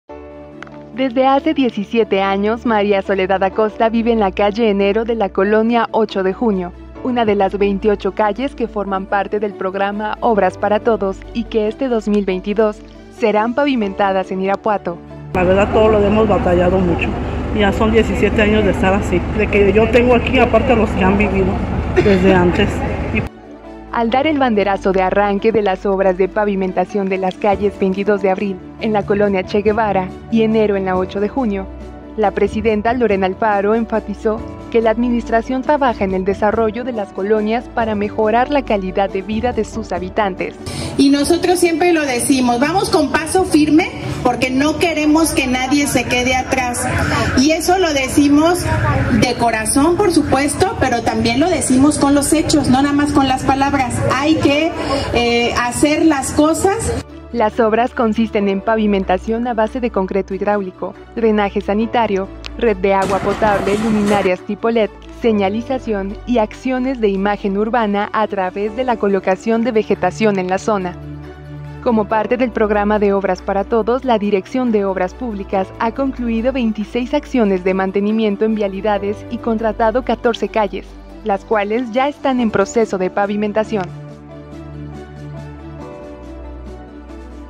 Arrancan más pavimentaciones de calles en Irapuato
Irapuato, Gto. (Opinión Bajío).- La calle Enero de la colonia 8 de Junio, en donde doña María vive desde hace 17 años, es una de las 28 calles que se pavimentarán con el programa ObrasParaTodos del Gobierno de Irapuato, en lo que resta de 2022.